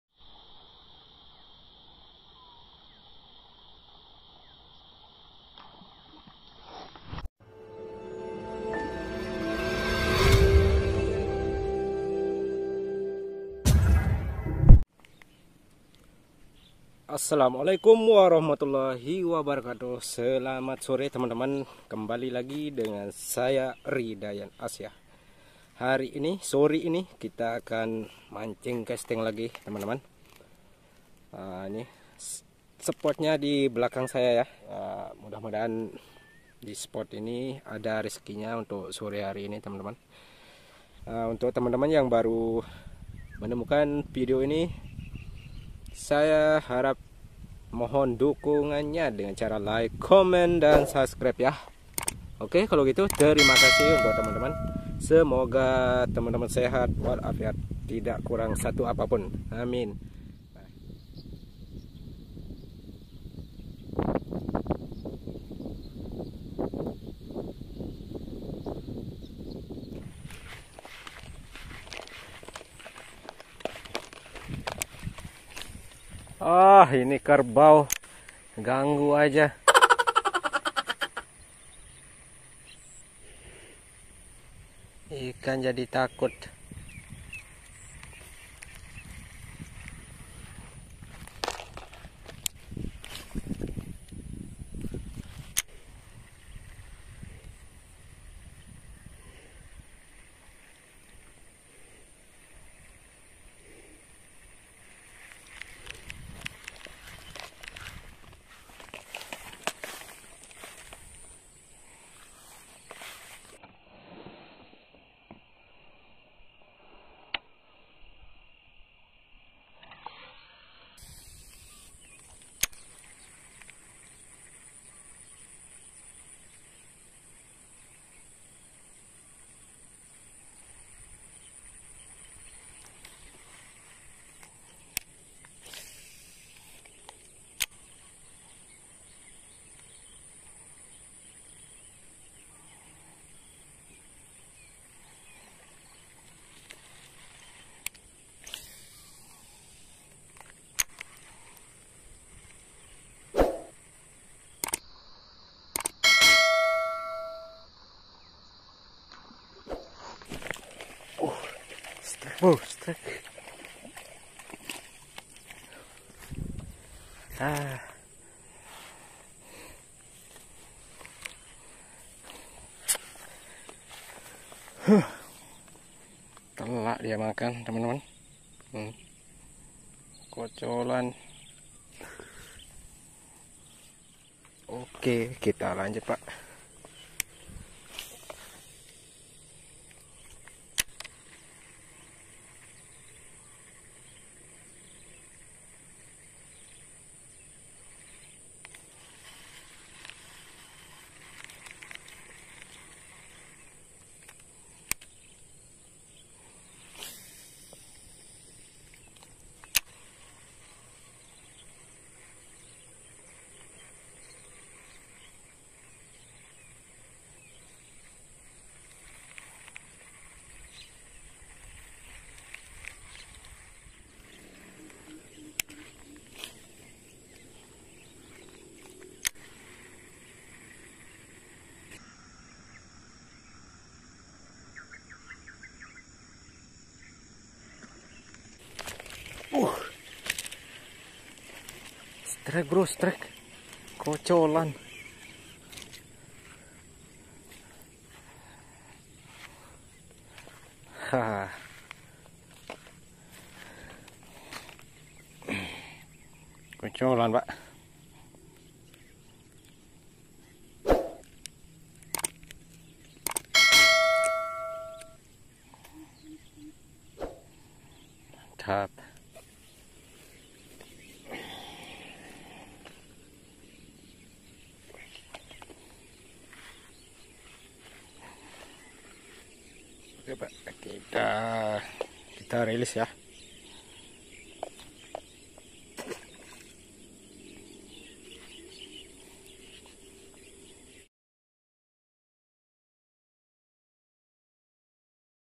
Mancing Ikan Haruan | waoo.‼️ada penampakan PREDATOR bersisik.❗
Mancing Ikan Haruan | waoo.‼️ada penampakan PREDATOR
bersisik.❗

Selamat datang sahabat mancing(ri dhayend asia)di sore hari ini saya mancing ikan haruan lagi.dan semoga sahabat semua bisa terhibur dengan adanya video mancing ikan haruan atau ikan gabus ini,,👍(TERIMA KASIH)